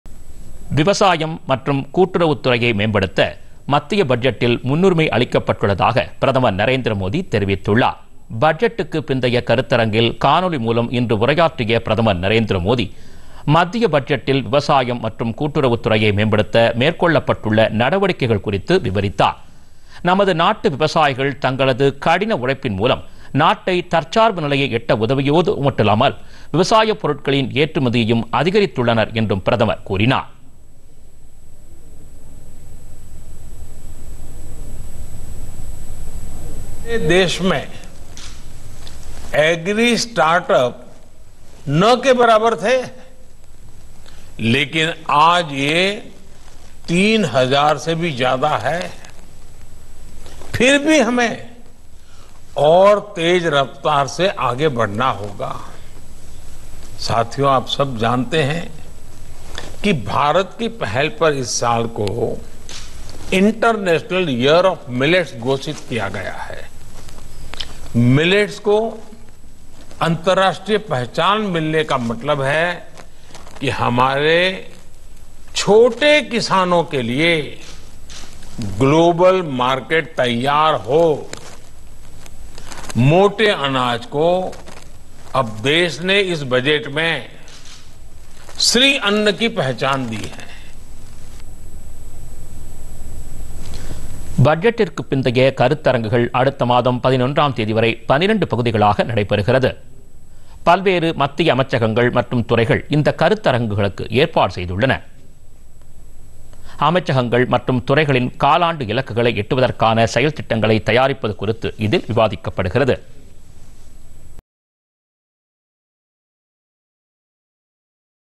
விவசாயம், கூட்டுறவுத்துறைக்கு முன்னுரிமை
விவசாயம், கூட்டுறவுத்துறையை மேம்படுத்த மத்திய பட்ஜெட்டில் முன்னுரிமை  - பிரதமர் நரேந்திர மோதி.